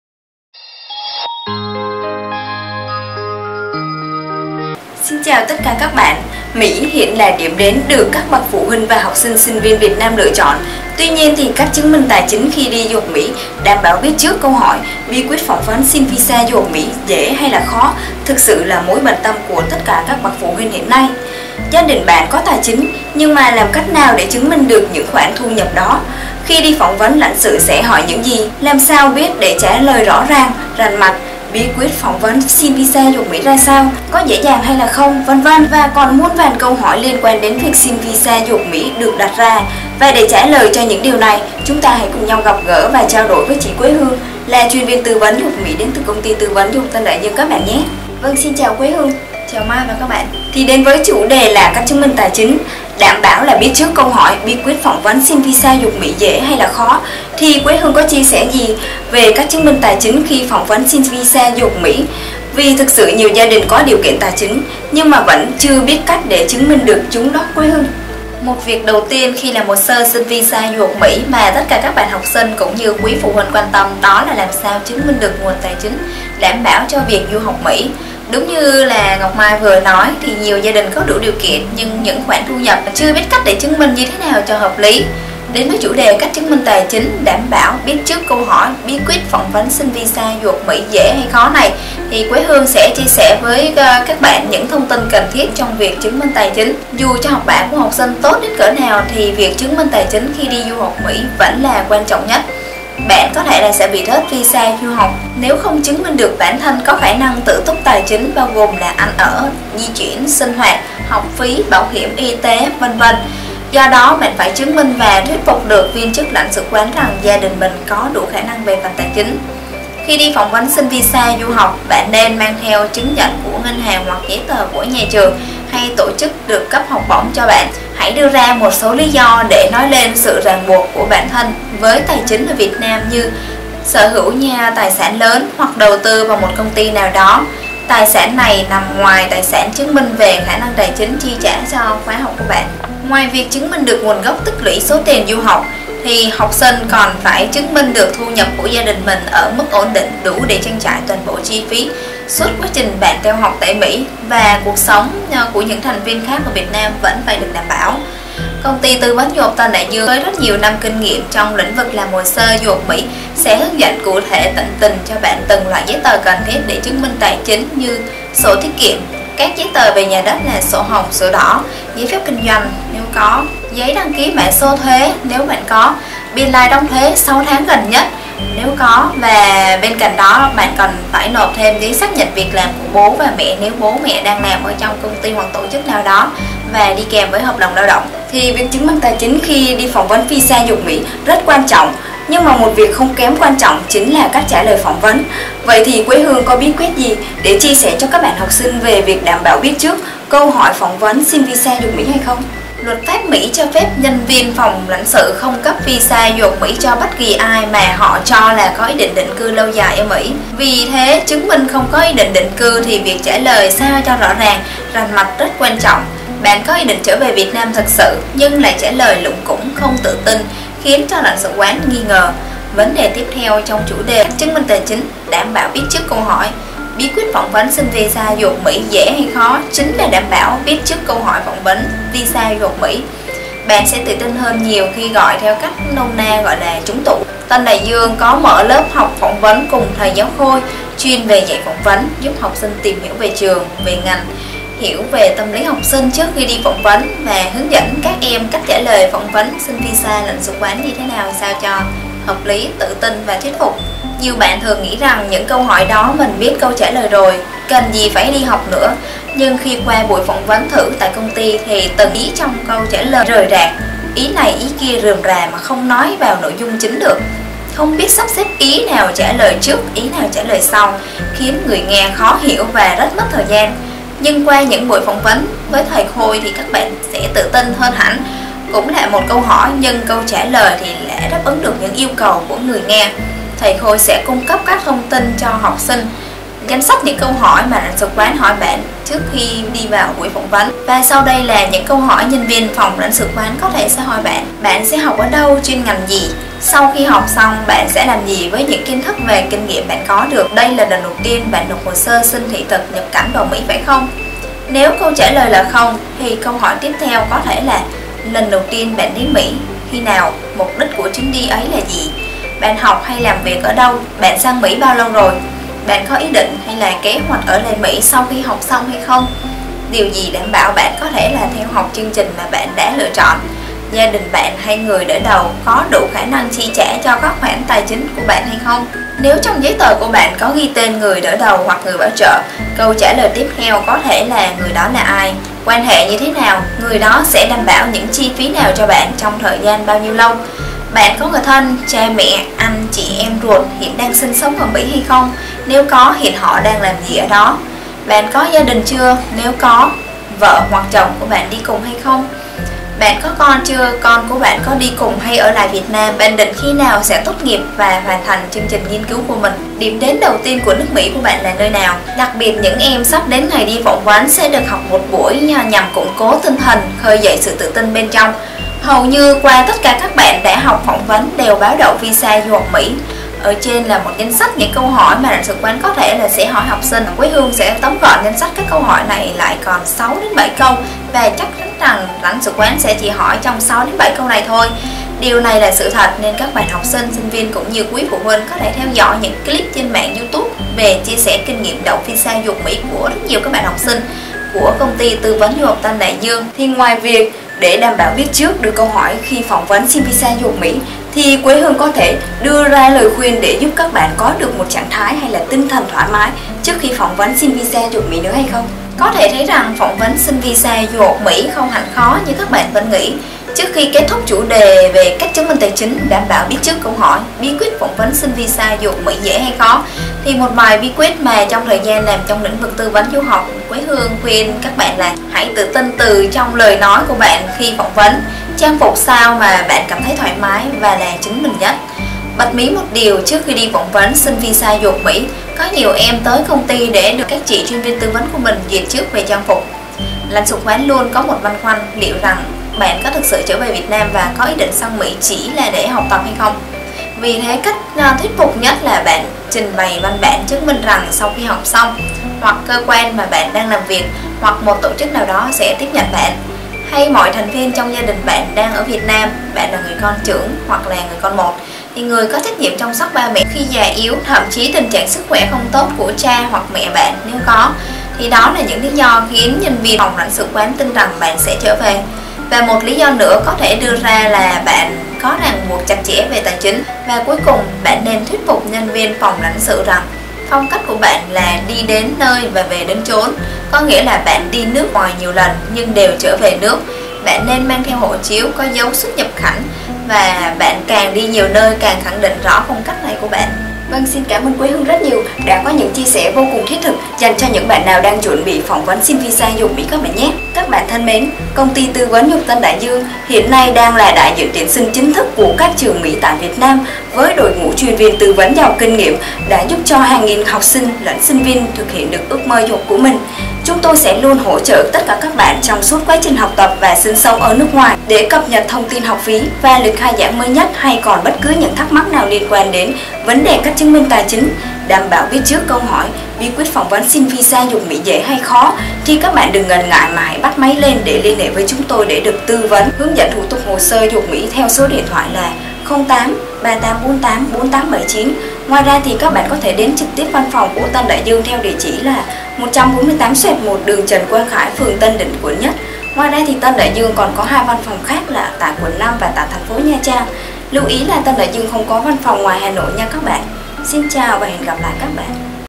Bí quyết chứng minh tài chính, biết trước câu hỏi phỏng vấn Visa Mỹ
Cách chứng minh tài chính, đảm bảo biết trước câu hỏi, bí quyết phỏng vấn xin visa du học Mỹ dễ dàng. Hãy cùng lắng nghe TDD chia sẻ cùng các bạn nhé!